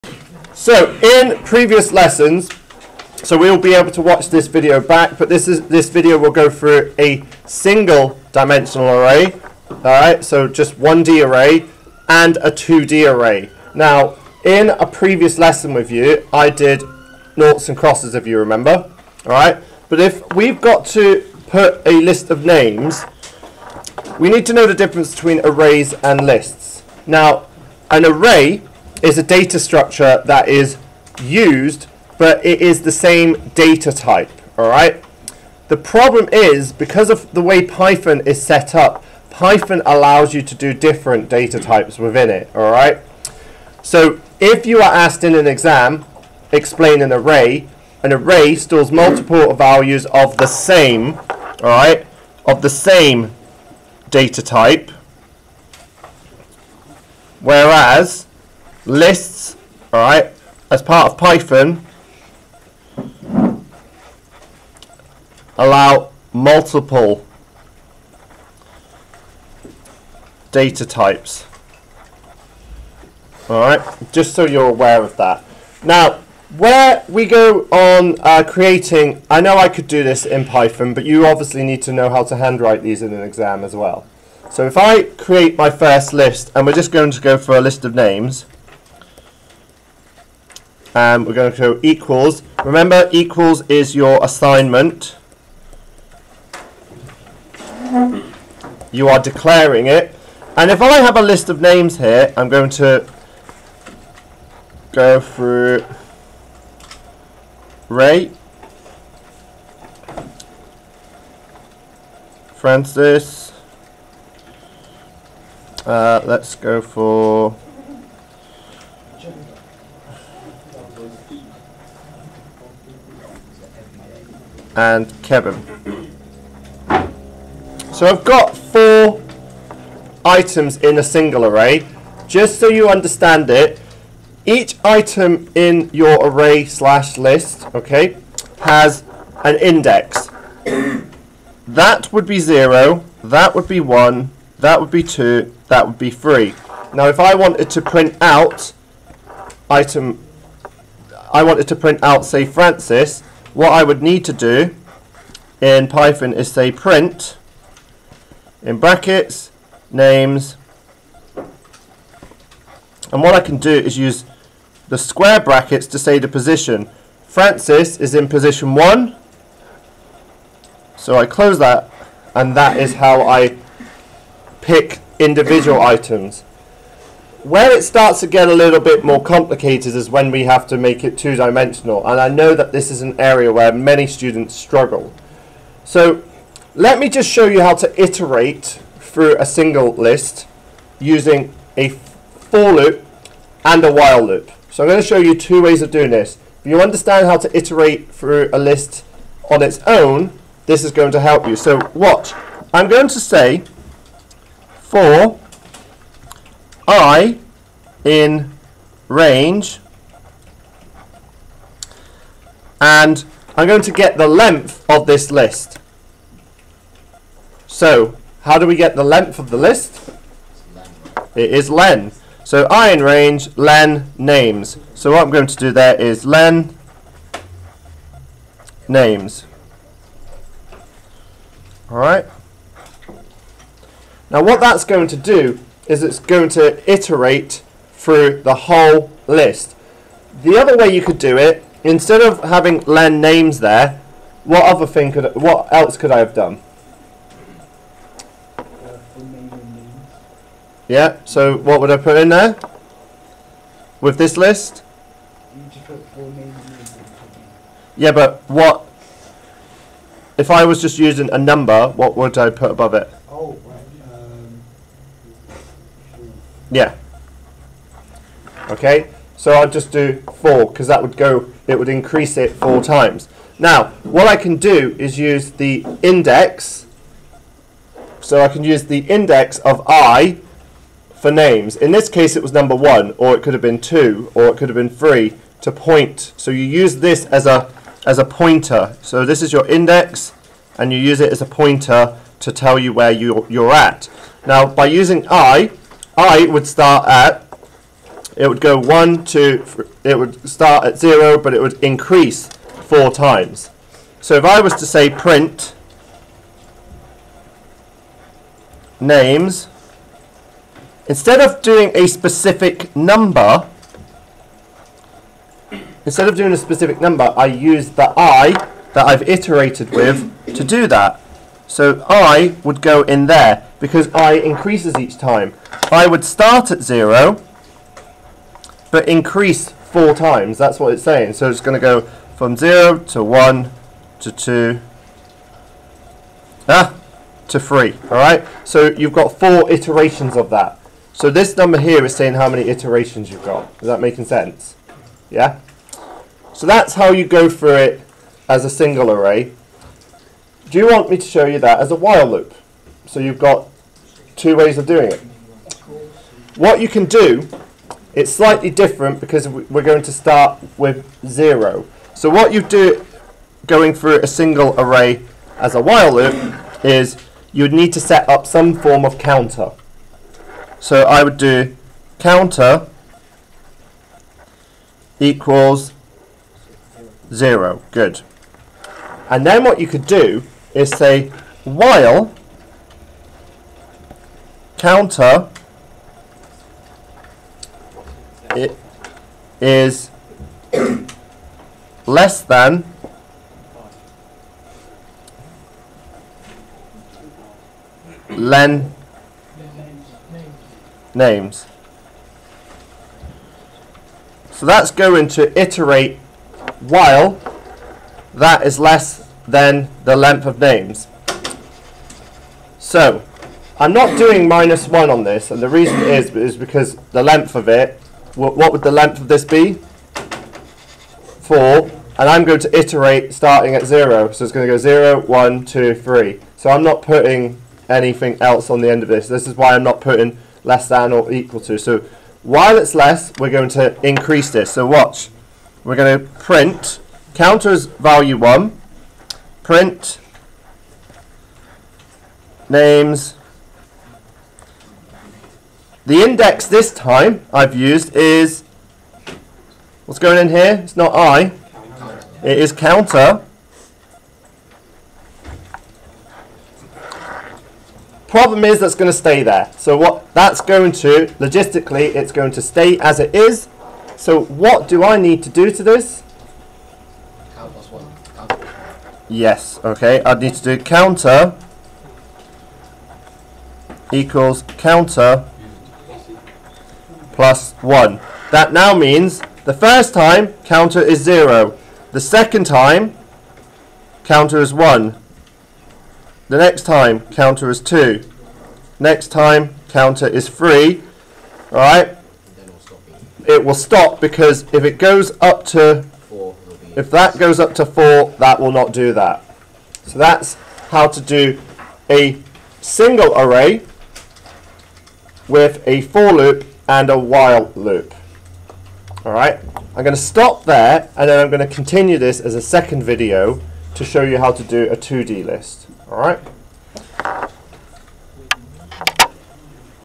List / Array - Python Explained (Single / 1D Array) - Iteration
A video tutorial explaining how to use Arrays and List in Python. It shows how the lists work and how to pick specific items. The video also shows you how to go through the list using a while or for loop.